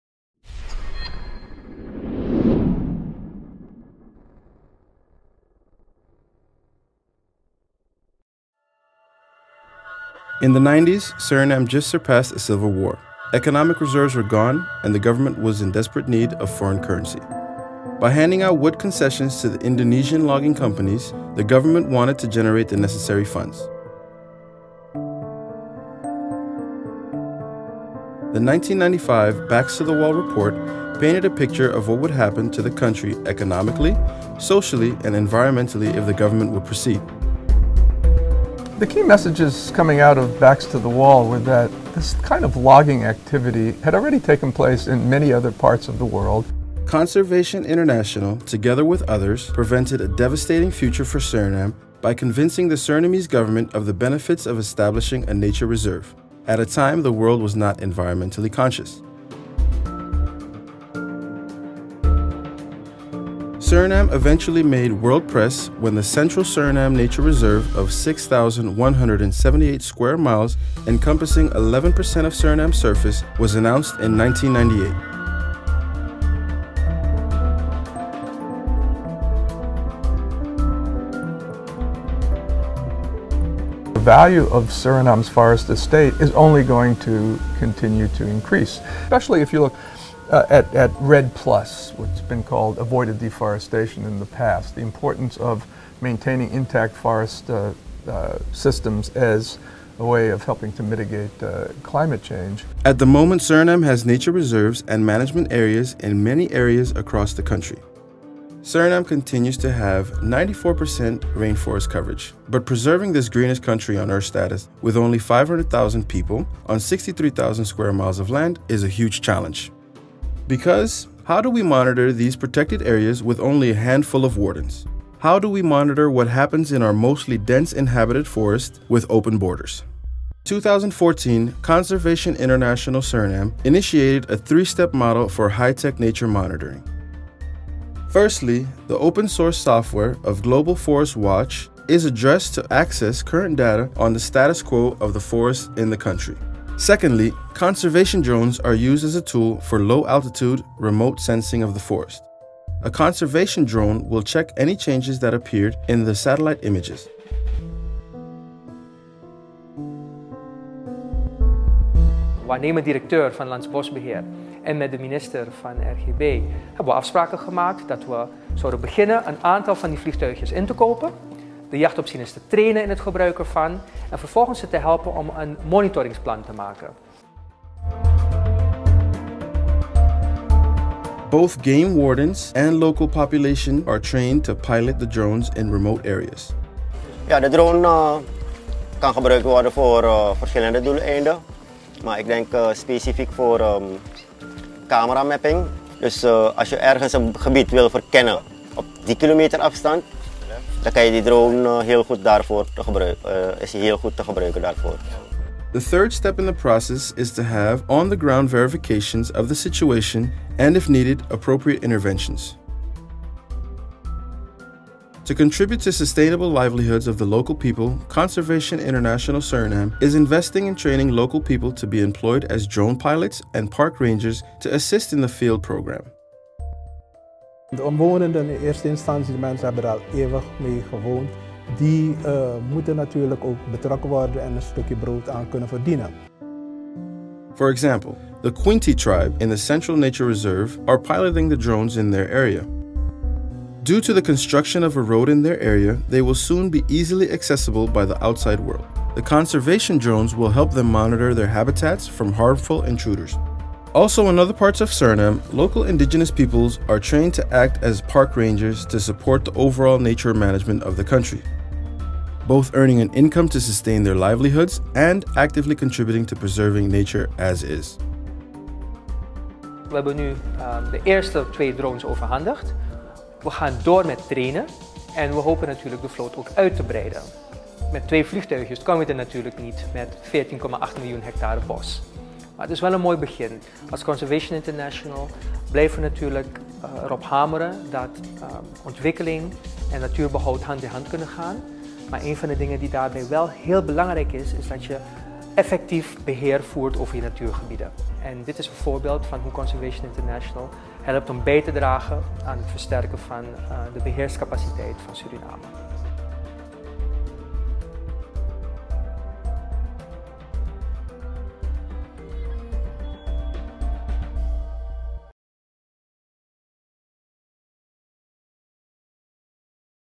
Nature Monitoring through innovative technology (drones)
Suriname has dedicated over 13% of its surface to nature reserves. This means almost 10 million hectares of pristine rainforest needs to be monitored. Conservation International Suriname developed a three tier program consisting of satellite data collection/analysis, the use of conservation drones and on the ground verification of indigenous park rangers. The three step approach will help the monitoring of  dense nature areas in the greenest country on earth.